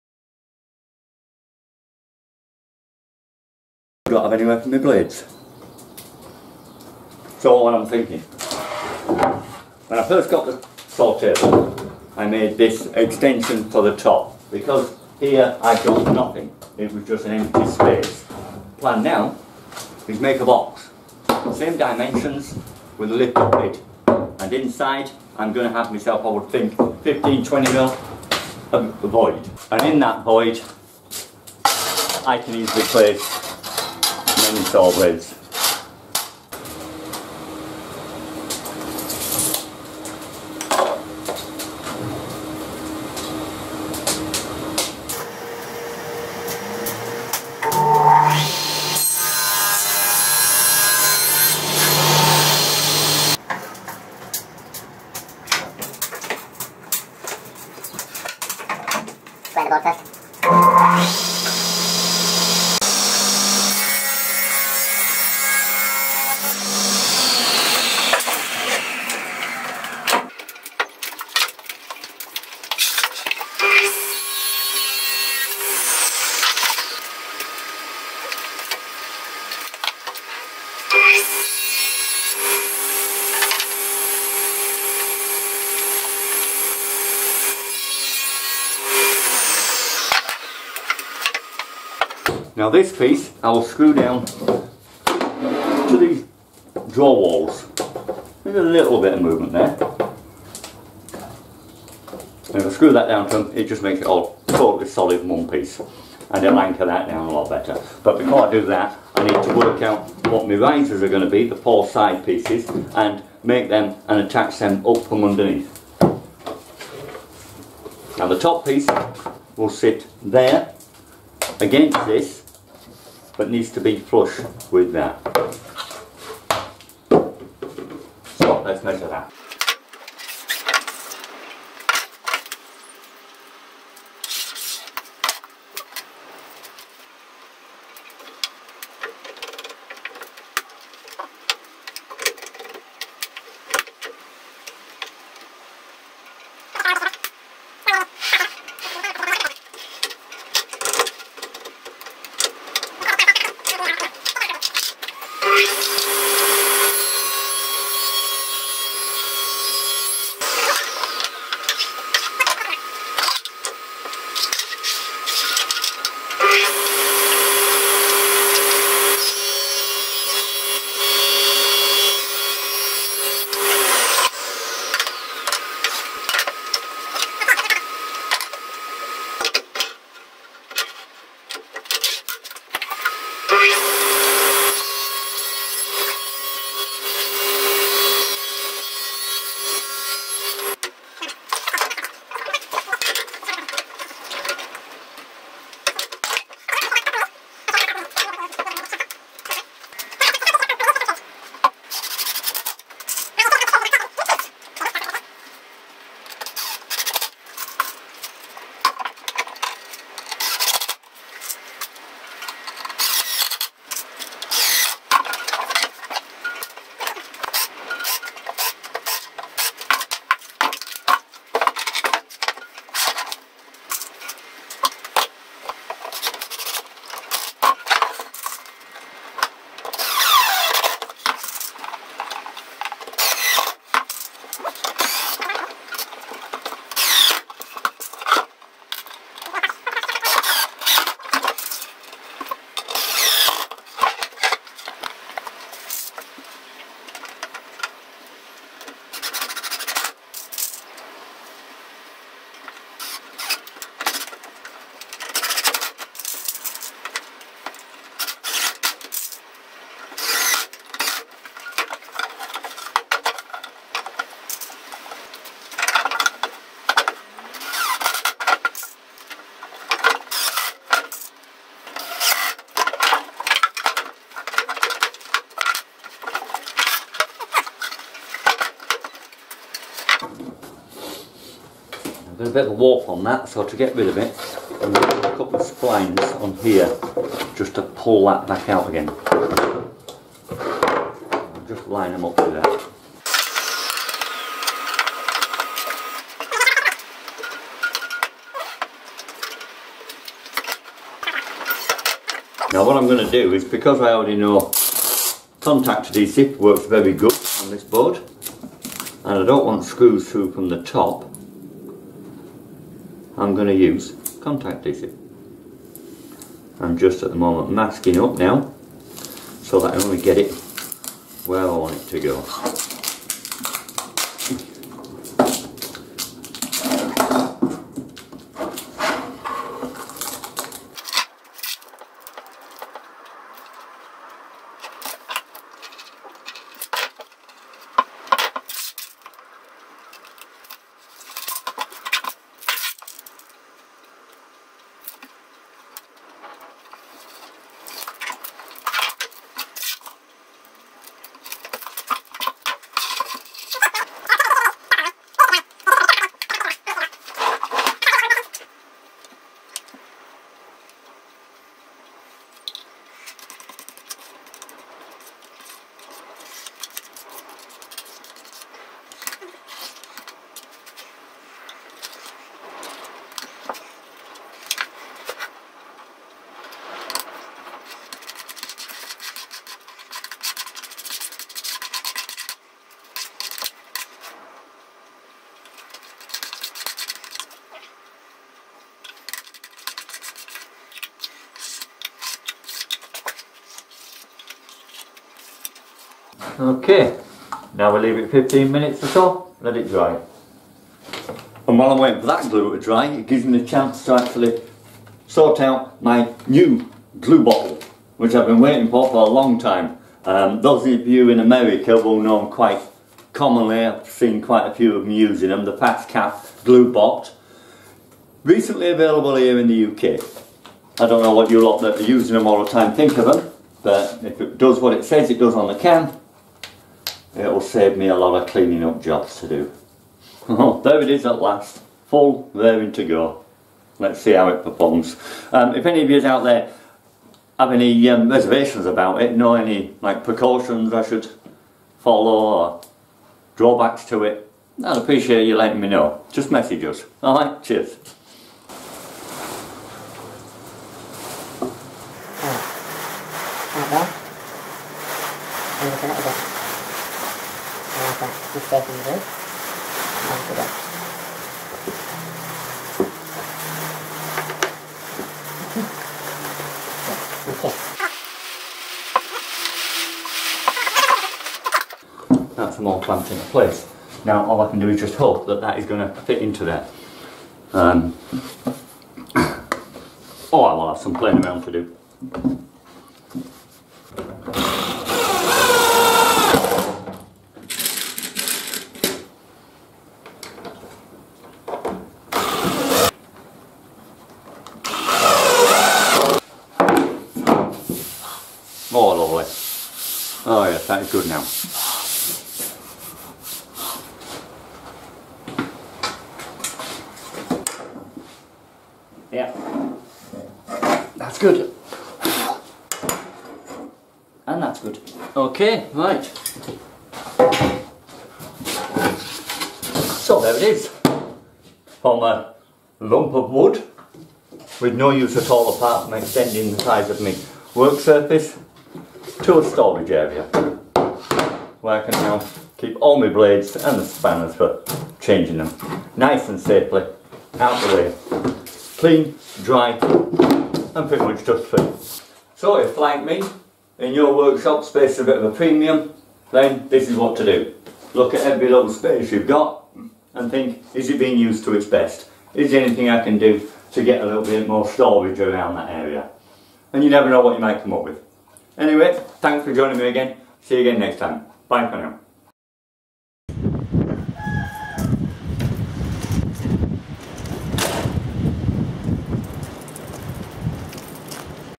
Saw Blade Storage Solution
finishing off with my saw table storage I am now adding the saw blade storage box as part of the worktop